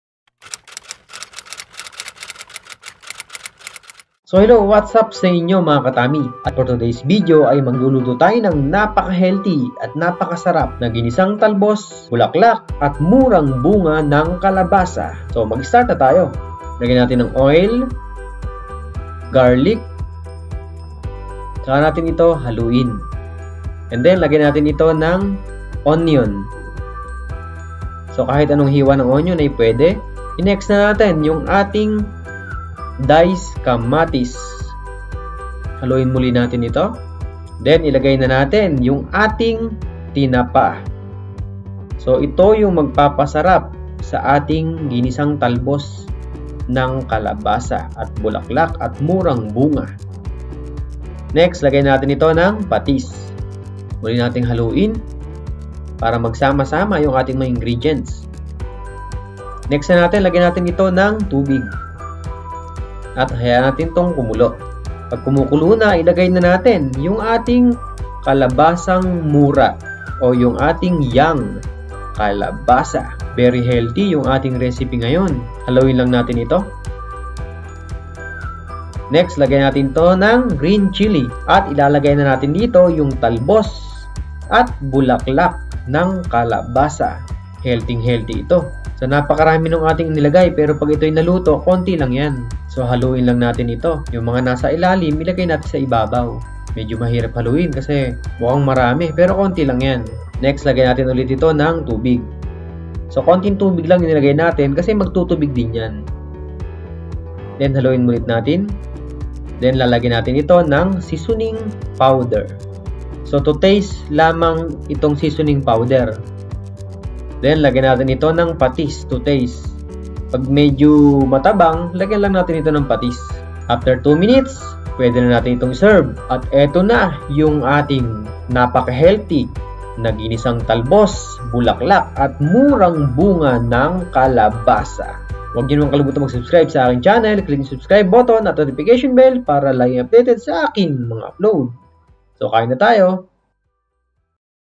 Ginisang Talbos, bulaklak at murang bunga ng kalabasa
Hello Mga ka TUMMY sa video na ito ipapakita ko sa inyo kung paano lutuin ang : Ginisang Talbos, bulaklak at murang bunga ng kalabasa